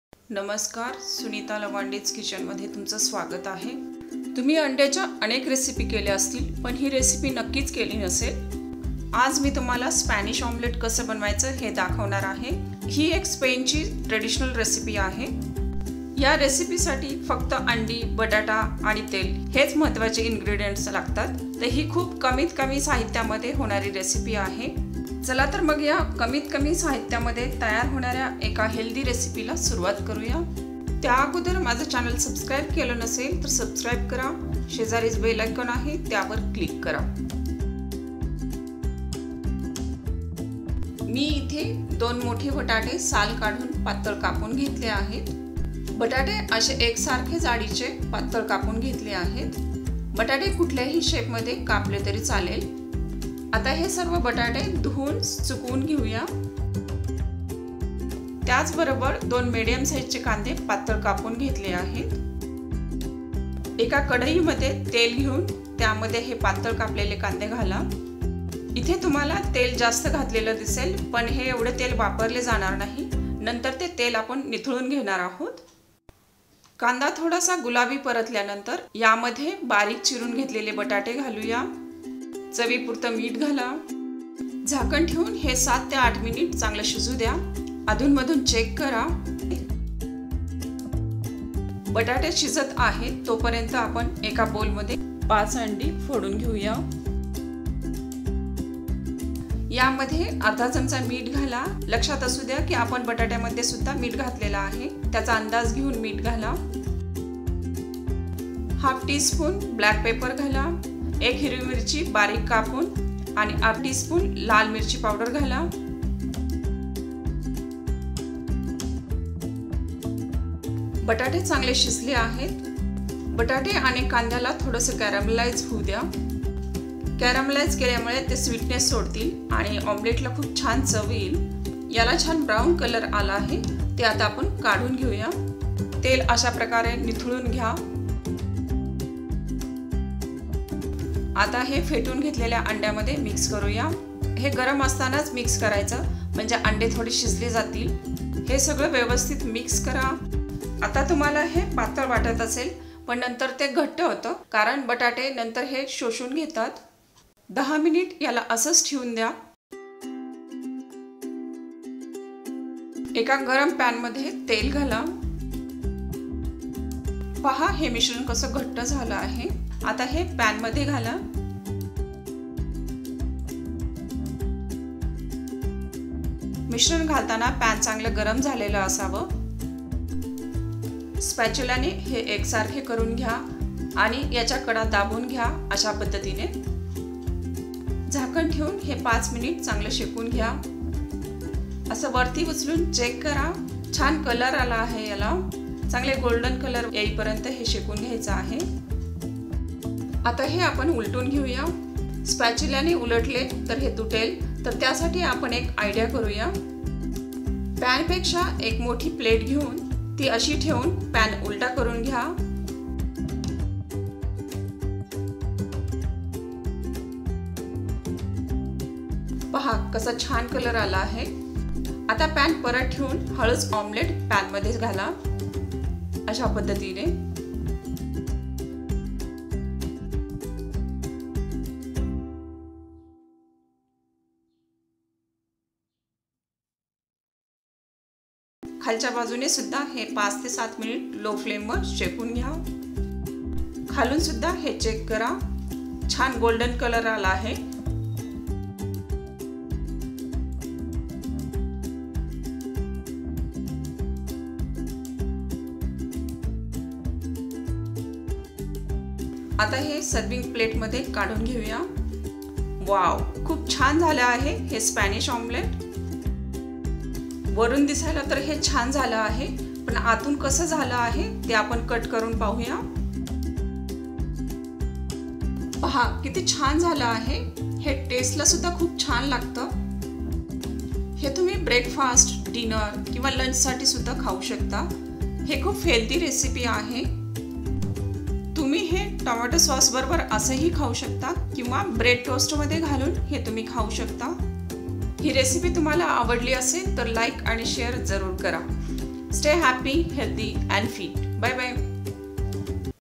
अंड्याचा आगळा वेगळा नाष्टा | Spanish Omelette | Potato Omelette
Hello,
Spanish Omelette. This is traditional recipe of Spain.It needs simple ingredients which are available at home.

Ingredient list.
Potato - 2,
Onion - 2,
Eggs- 5,
Oil,